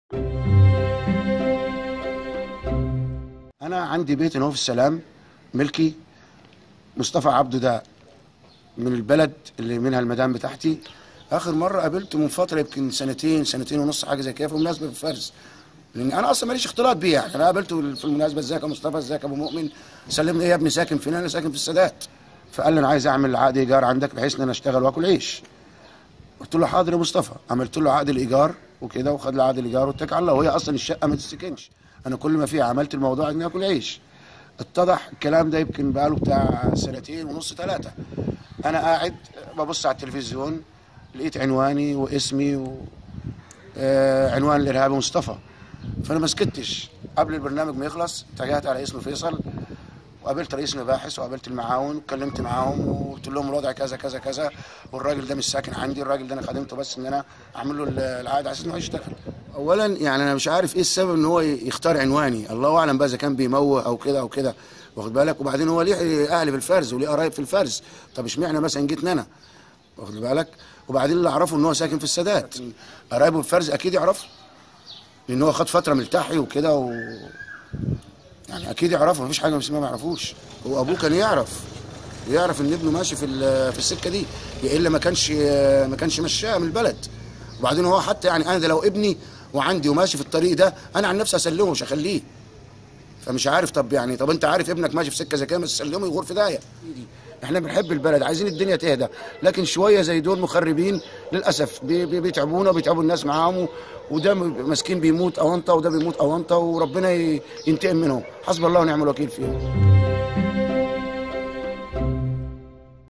مالك «شقة» متهم في تفجير طنطا : لو إبني كنت سلمته
تقرير: حسام الدين أحمد
مونتاج : اسلام فرغلي